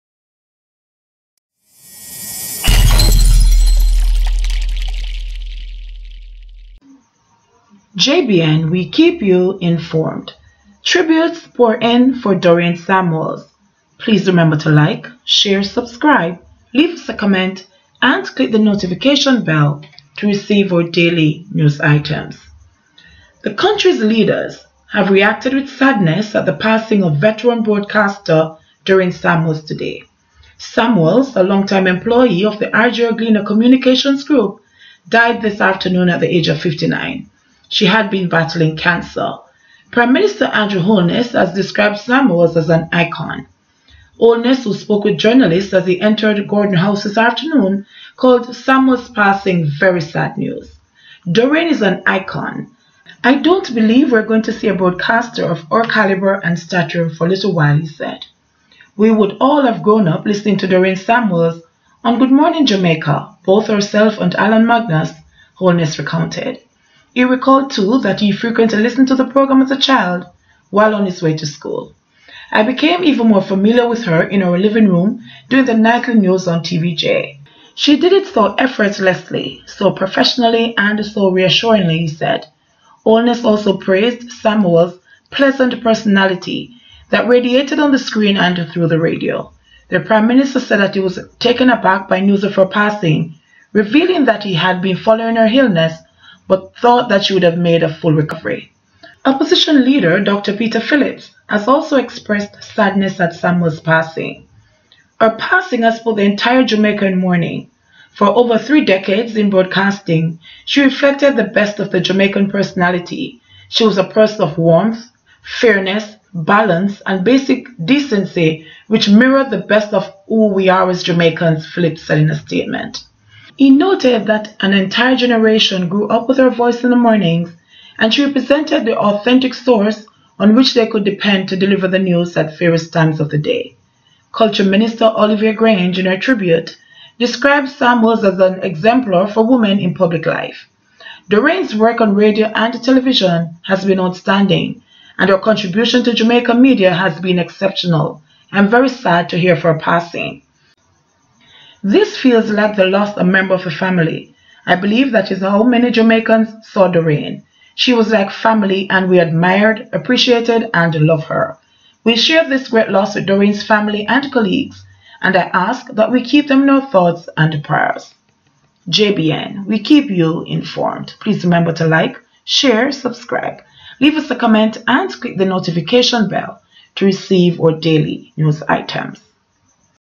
Dorraine Samuels De@d She Will Be M!ssed/JBN
The country’s leaders have reacted with sadness at the passing of veteran broadcaster Dorraine Samuels today.

Samuels, a long-time employee of the RJRGLEANER Communications Group, died this afternoon at the age of 59.

She had been battling cancer.

Prime Minister Andrew Holness has described Samuels as an icon.
Holness, who spoke with journalists as he entered Gordon House this afternoon, called Samuels' passing "very sad news".

"Dorraine is an icon. I don't believe we are going to see a broadcaster of her calibre and stature for a little while," he said.

"We would all have grown up listening to Dorraine Samuels on 'Good Morning Jamaica', both herself and Allan Magnus," Holness recounted.

He recalled, too, that he frequently listened to the programme as a child while on his way to school.

"I became even more familiar with her in our living room doing the nightly news on TVJ. She did it so effortlessly, so professionally, and so reassuringly," he said.

Holness also praised Samuels' "pleasant personality that radiated on the screen and through the radio".

The prime minister said that he was taken aback by news of her passing, revealing that h had been following her illness but thought that she would make a full recovery.

Opposition leader, Dr Peter Phillips, has also expressed sadness at Samuels’ passing.

“Her passing has put the entire Jamaica in mourning. For over three decades in broadcasting, she reflected the best of the Jamaican personality, she was a person of warmth, fairness, balance and basic decency which mirrored the best of who we are as Jamaicans,” Phillips said in a statement.

He noted that an entire generation grew up with her voice in the mornings and she represented the authentic source on which they could depend to deliver the news at various times of the day.

Culture Minister Olivia Grange, in her tribute, described Samuels as “an exemplar for women in public life.”

“Dorraine’s work on radio and television has been outstanding and her contribution to Jamaican media has been exceptional. I am very sad to hear of her passing.

“This feels like the loss a member of the family. I believe that is how many Jamaicans saw Dorraine — she was like family and we admired, appreciated and loved her. We share this great loss with Dorraine’s family and colleagues and I ask that we keep them in our thoughts and prayers.”